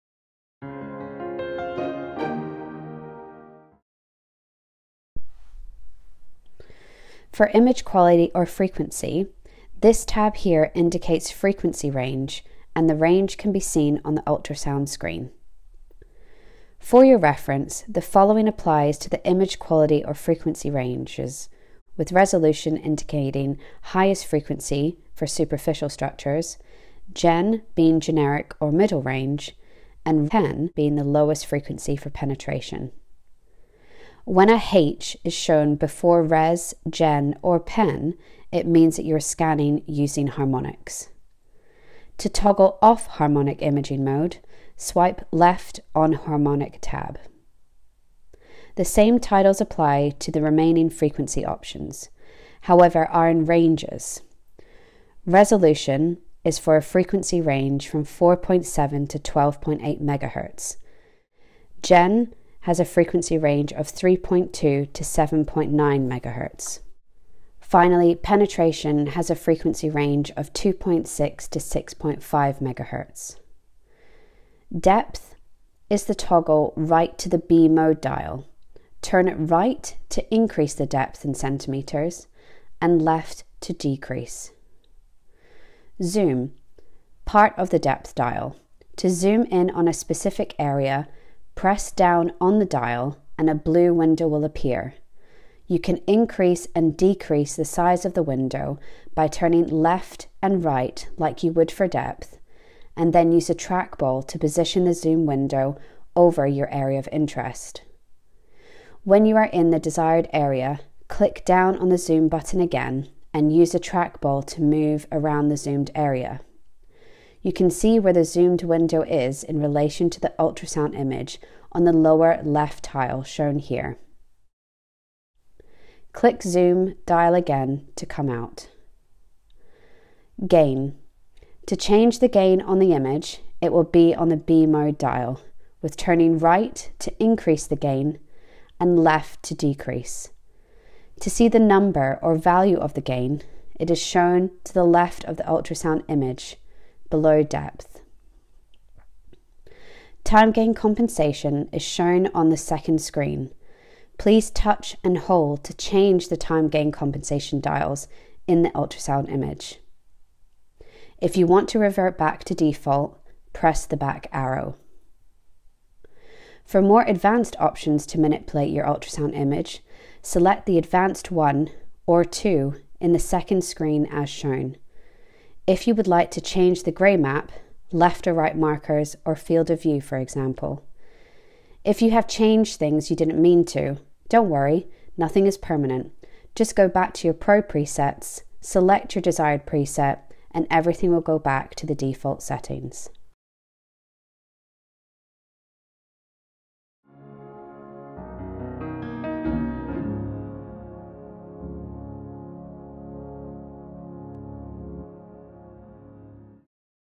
Mindray Vetus 50: How to use the Basic B Mode Settings for Image Optimisation
🐾 Welcome back to our Vetus 50 Scanner Mastery Series! 🐾 Get ready to elevate your scanning game with our second installment, where we delve into mastering image quality and frequency settings.

🔍 Explore the Image Quality/Frequency tab and learn to decipher frequency ranges effortlessly. We'll guide you through toggling off Harmonic imaging mode and understanding the nuances of resolution, generic, and penetration frequencies.

💡 Key Highlights:
- Discover the Harmonic Imaging modes and their corresponding frequencies.
- Learn to adjust depth and zoom settings for precise scanning.
- Master gain control and TGC manipulation for optimal image clarity.
- Uncover advanced options like Gray Map adjustments and FOV selection.

📹 Join us as we demystify the intricacies of image quality and frequency settings on the Vetus 50 scanner!